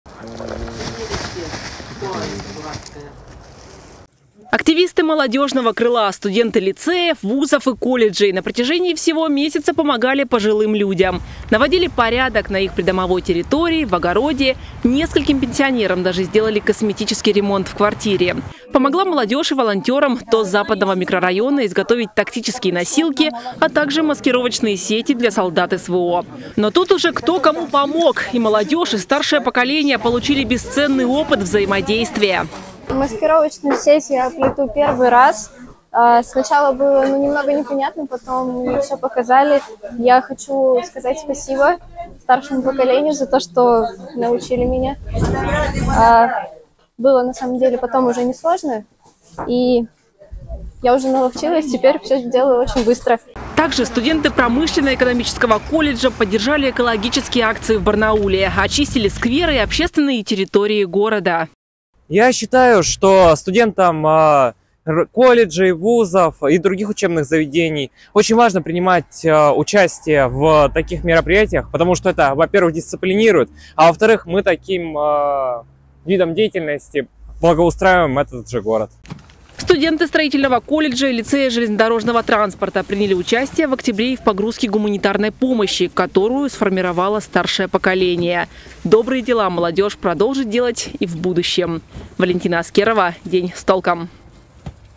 Барнаульские студенты присоединились к месячнику пожилого человека
Навели порядок на придомовой территории, сделали ремонт и сплели маскировочные сети. Барнаульские студенты присоединились к месячнику пожилого человека и провели серию мероприятий для пенсионеров.  Подробнее о добрых делах общественников, представителей Молодежного крыла Народного фронта – смотрите в сюжете.